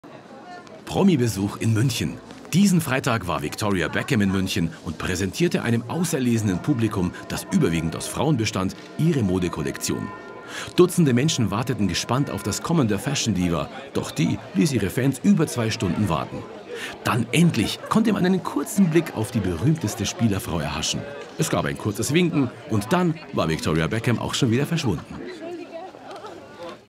Victoria Beckham in München - TV BAYERN LIVE* (17.11.2013)
Promi Besuch in München:
Diesen Freitag war Viktoria Beckham in München und präsentierte ausgelesenen Personen, überwiegend Frauen, ihre Modekollektion. Mehrere Fans warteten gespannt auf das Kommen der Fashion Diva.
Doch die ließ ihre Fans über 2 Stunden warten. Dann endlich konnte man einen kurzen Blick auf  die berühmteste Spielerfrau erhaschen.
Es gab ein kurzes Winken und dann war Viktoria Beckham auch schon wieder verschwunden.